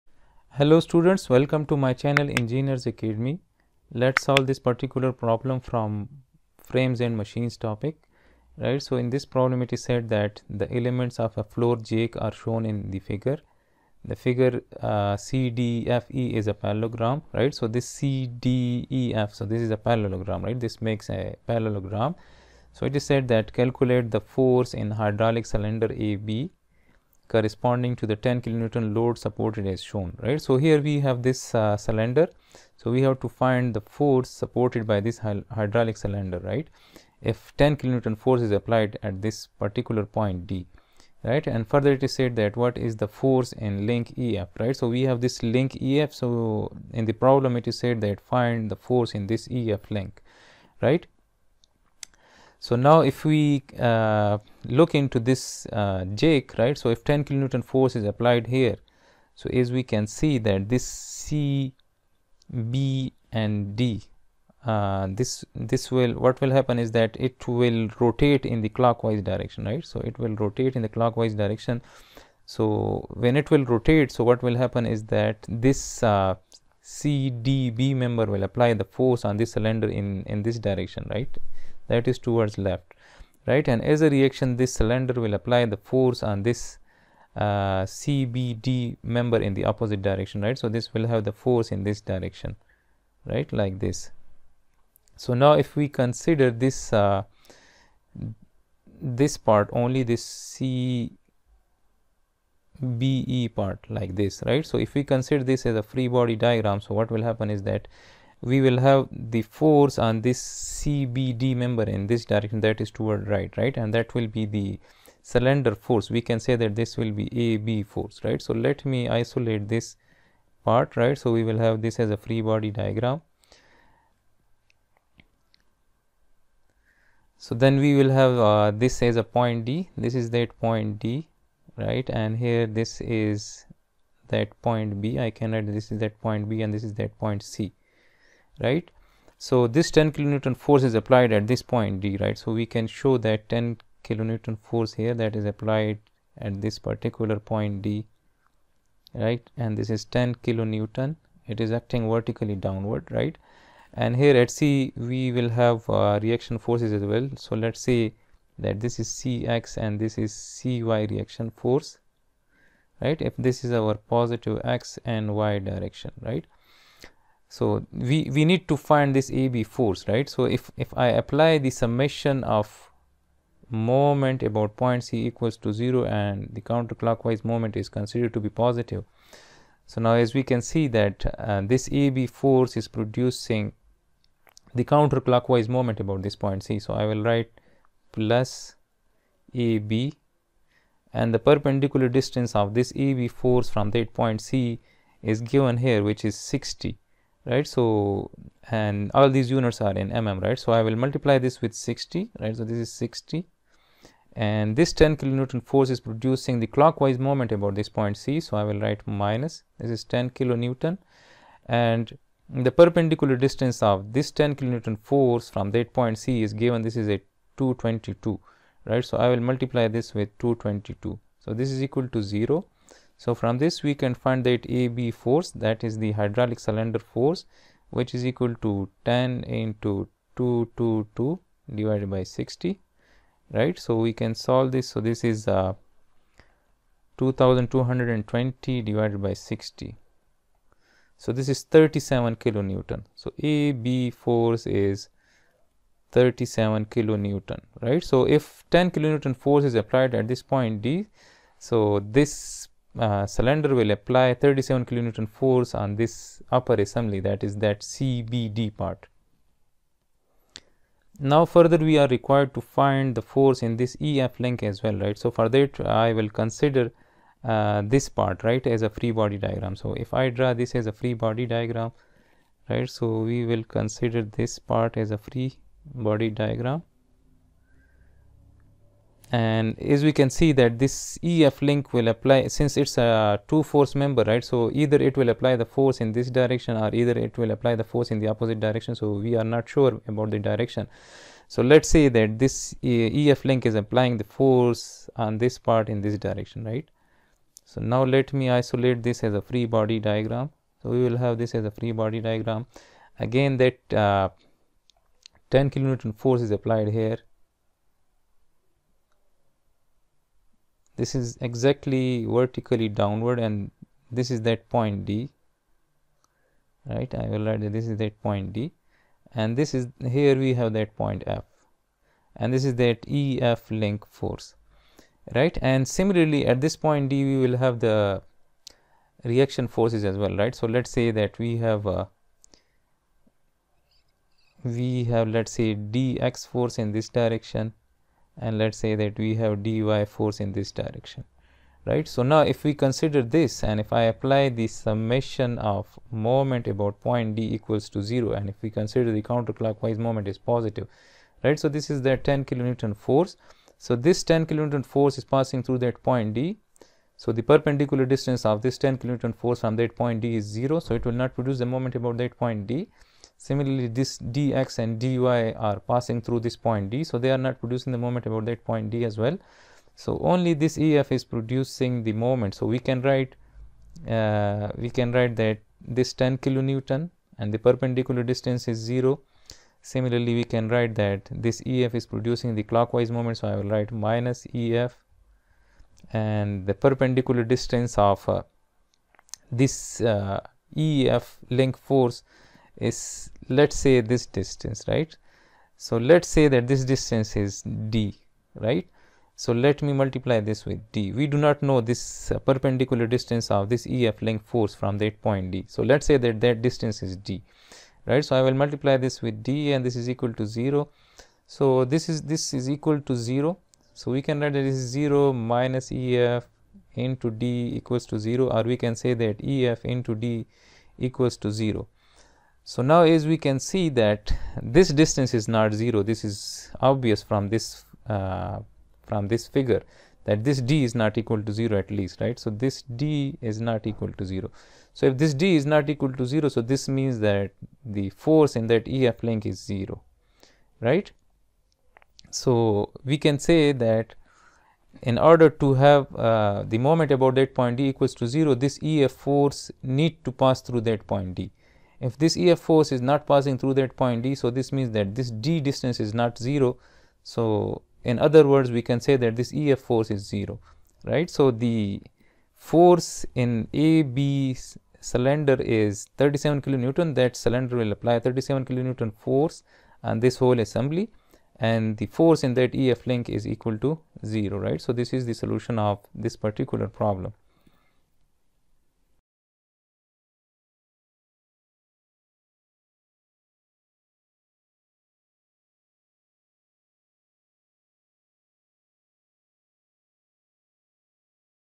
Calculate the force in the hydraulic cylinder AB. Frames and Machines | Engineers Academy
Chapter 4: Structures
Frames and Machines
How to Solve Frames and Machines Problems Statics

4/80 The elements of a floor jack are shown in the figure. The figure CDFE is a parallelogram. Calculate the force in the hydraulic cylinder AB corresponding to the 10-kN load supported as shown. What is the force in link EF ?